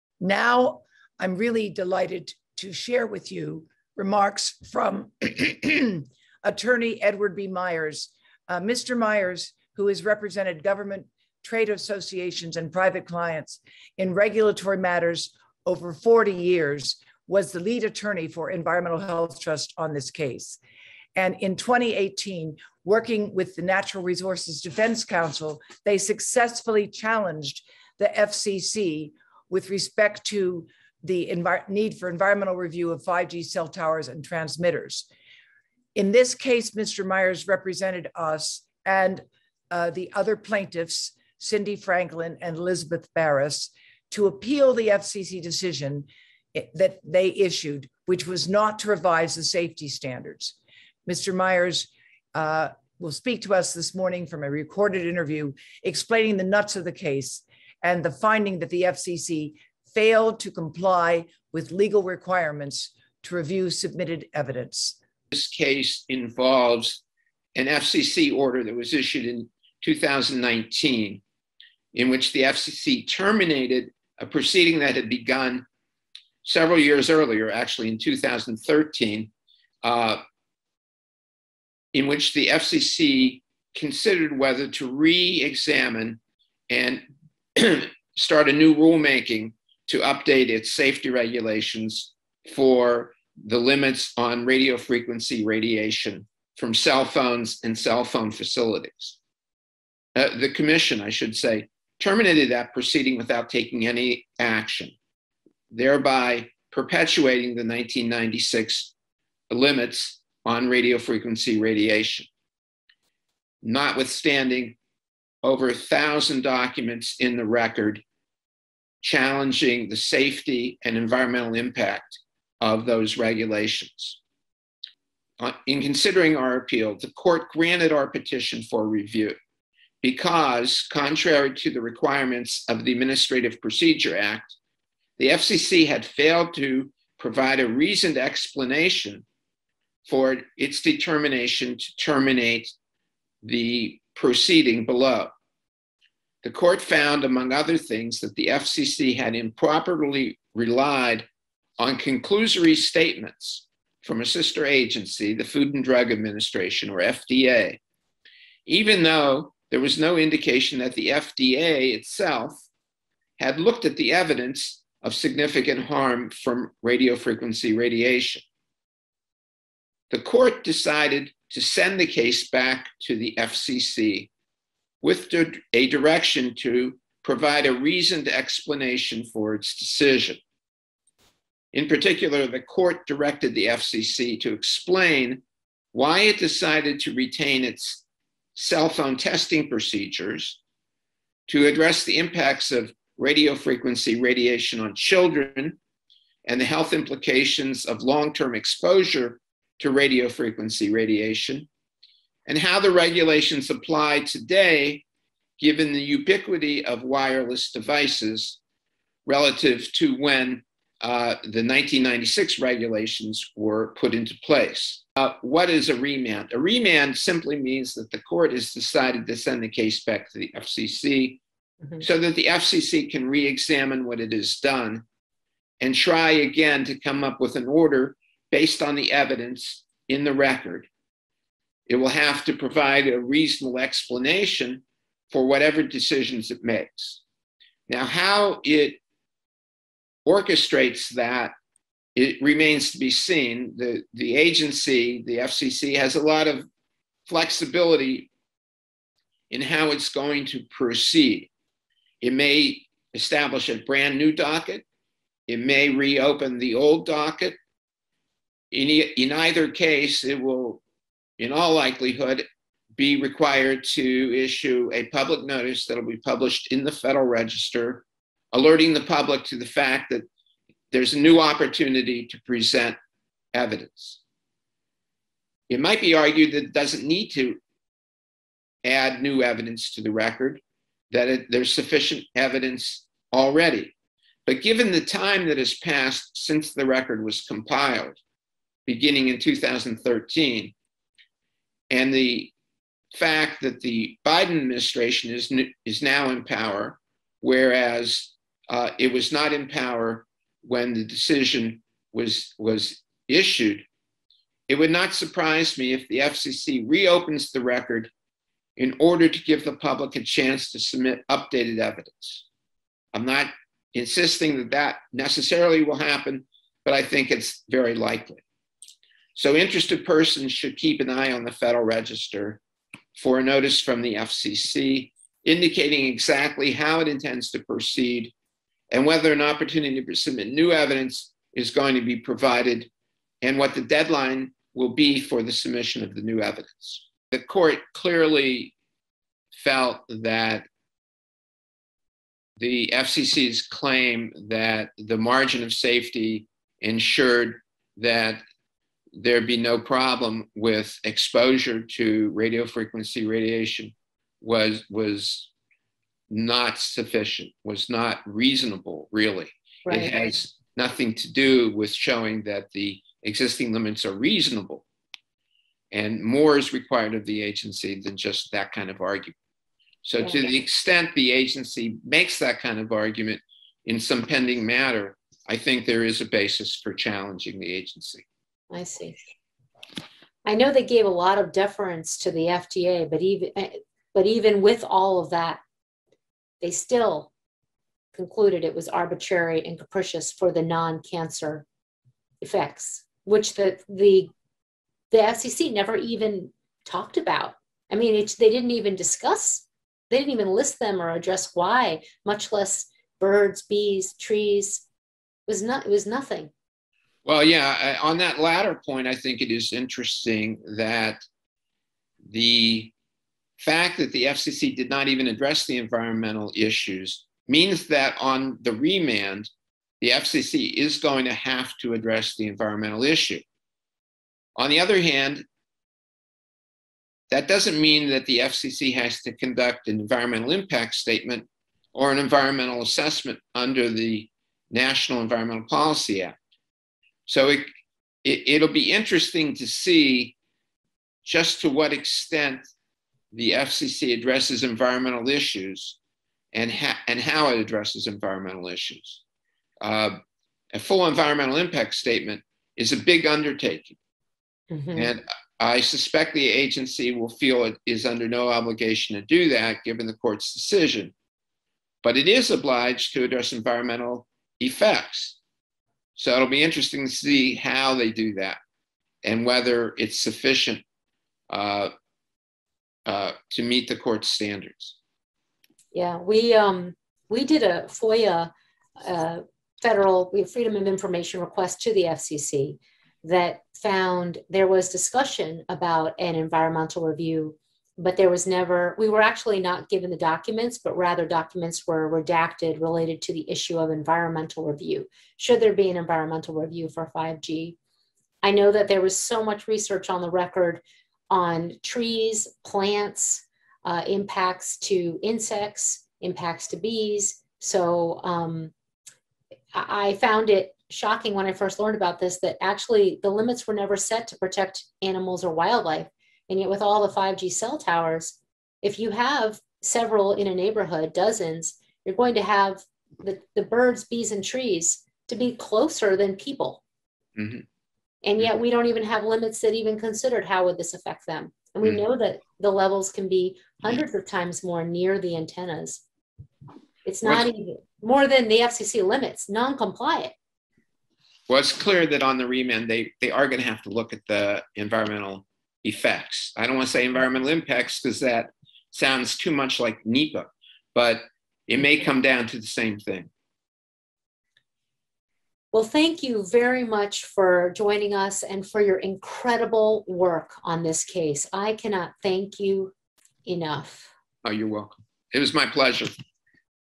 EHT v. FCC Lawsuit | Press Conference Excerpts of Edward B. Myers, EHT attorney
In an historic decision, the U.S. Court of Appeals for the District of Columbia Circuit judged in favor of environmental health groups and petitioners on Aug. 13, 2021, finding the Federal Communications Commission (FCC) violated the Administrative Procedure Act and failed to respond to comments on environmental harm.

The court ruled that the decision by the FCC to retain its 1996 safety limits for human exposure to wireless radiation was “arbitrary and capricious.”  and held that the FCC failed to respond to “record evidence that exposure to RF radiation at levels below the Commission’s current limits may cause negative health effects unrelated to cancer.”

Further, the agency demonstrated “a complete failure to respond to comments concerning environmental harm caused by RF radiation.” The court found the FCC ignored numerous organizations, scientists  and medical doctors who called on them to update limits and the court found the FCC failed to address these issues:

On August 16, 2021, Environmental Health Trust held a Press Conference with Devra Davis PhD, MPH, EHT president; Dr. Hugh Taylor MD Chair of Obstetrics, Gynecology and Reproductive Sciences, Yale School of Medicine and Yale-New Haven Hospital and president of American Society of  Reproductive Medicine; Edward B. Myers, EHT attorney; Theodora Scarato MSW, EHT Executive Director and a petitioner in the case; Frank Clegg, CEO of Canadians for Safe Technology and former President of Microsoft Canada; Paul Ben Ishai PhD, professor of physics at Ariel University in Israel; Cindy Franklin, founder of Consumers for Safe Cell Phones and a petitioner in the case, and Elizabeth Barris, director of the Peoples Initiative Foundation and a petitioner in the case.

Human exposure guidelines for wireless radiation were last set in 1996.